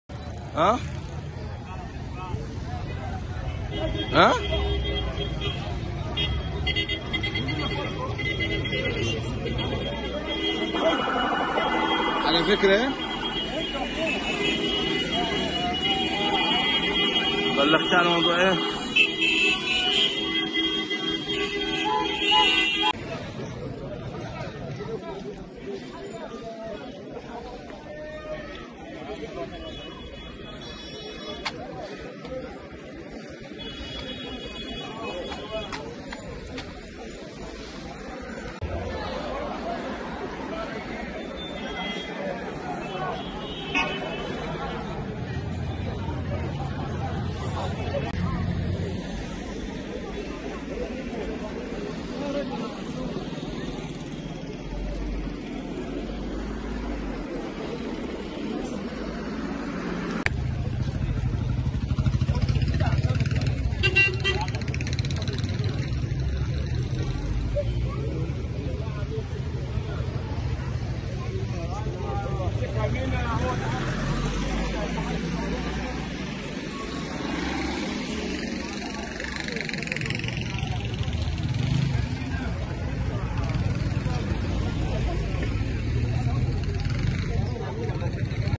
توافد المواطنين بكثافة على لجان الأقصر للإدلاء باصواتهم
ما عليك سوى الانتساب إلى هذه القناة للاستفادة من المزايا:
ElBaladOfficial#
ElBalad_Official#